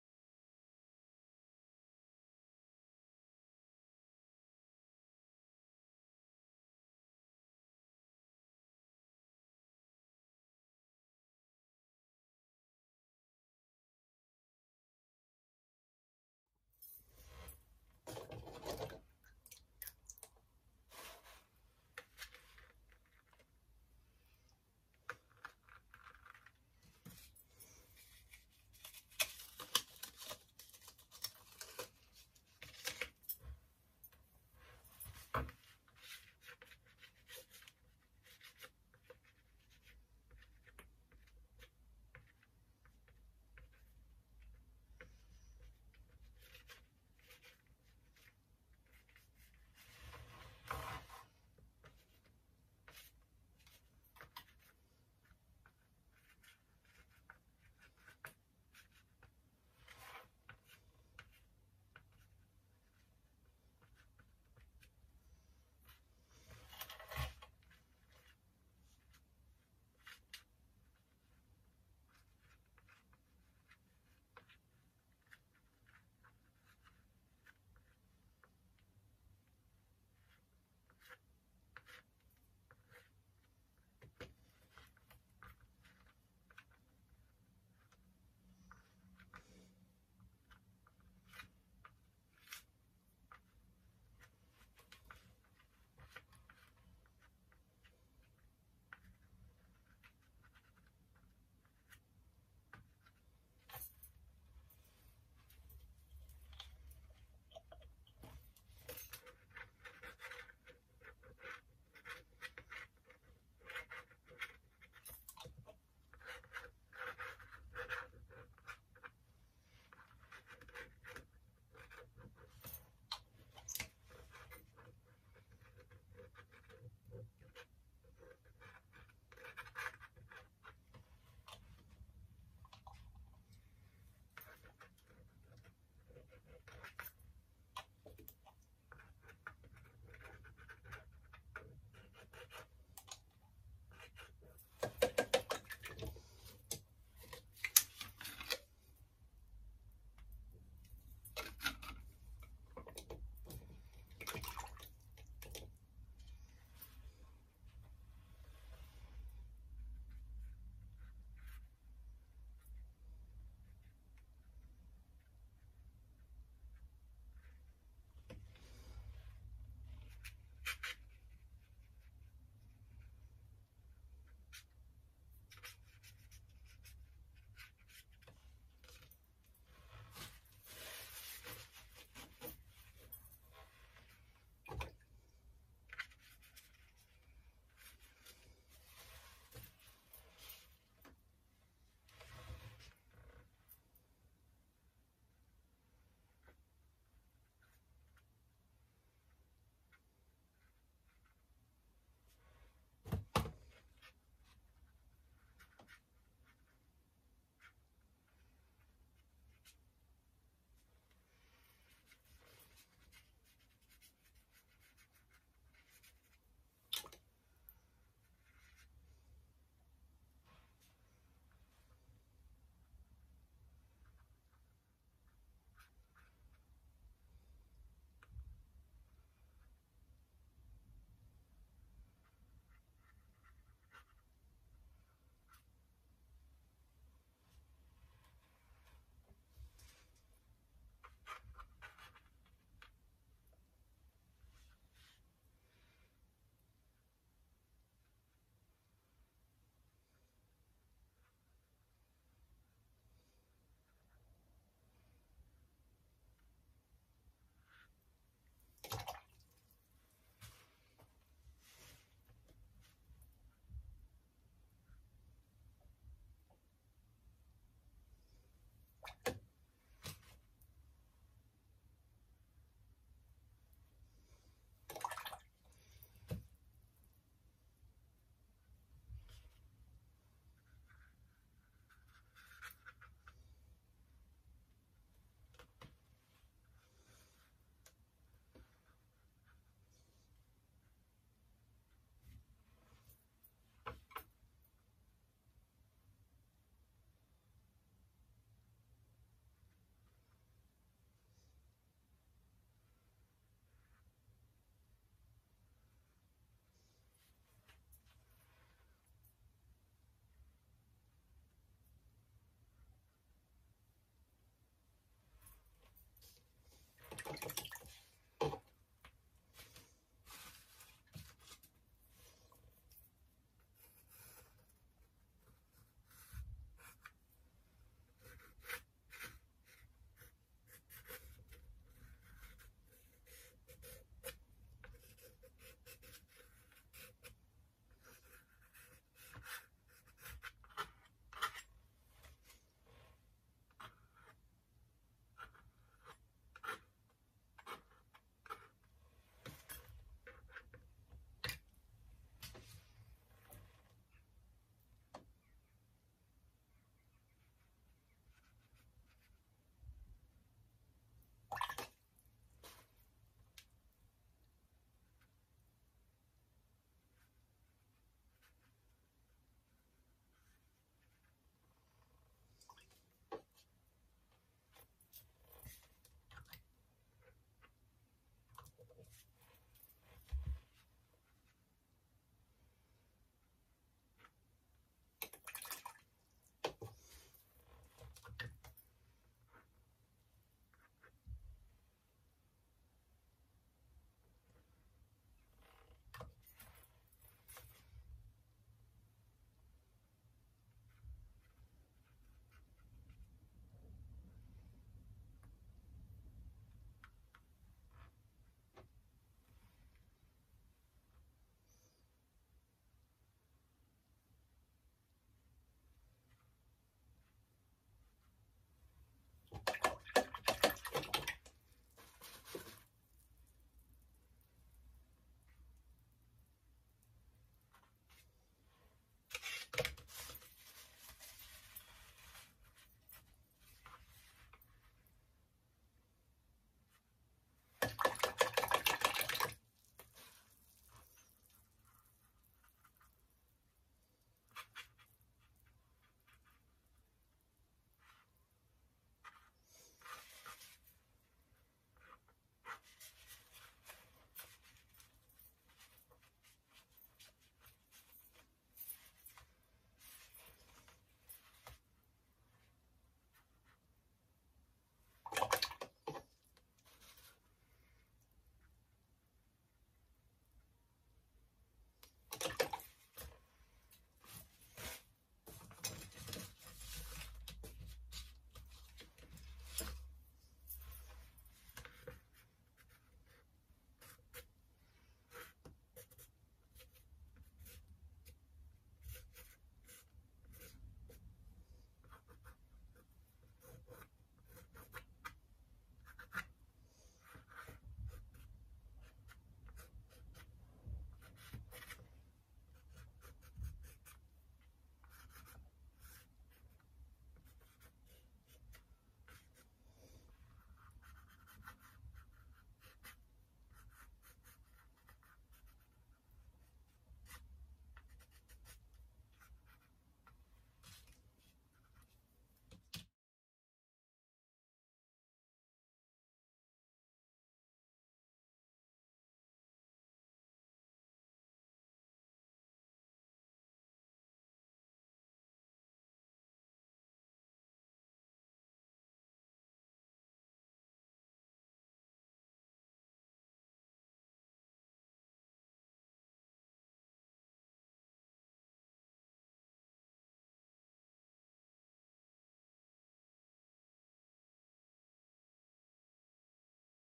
Warm Up Exercises Using Art Graf & Ink
After a mammoth weekend of trimming, cropping, cutting and mounting work ready for the upcoming Art & Yoga event, I had only intended going into the studio to have a tidy up...but...

It felt ages since I had created - only last week, but go with it - so I decided to just play around with the Art Graf Tailor's chalk and some ink...

I was channelling the reeds and the river, along with the fenlands... trying to get energy into the mark making...

Out of the twelve small squares, one was a complete disaster; I quite liked four - the ones where I hadn't overly 'fiddled', what a surprise - not!!

I think I should make signs of the phrases; 'intuitive - don't overthink', 'stop fiddling' & 'less is more' - and stick in a prominent position in front of me!!

I used:
Art Graf Tailors Chalk black pigment block
Daler Rowney FW Sepia Acrylic Ink
Winsor & Newton Granulation fluid
Pencil
Copper watercolour pencil
220gsm Seawhite of Brighton cartridge paper
Jacksons own brand small dagger brush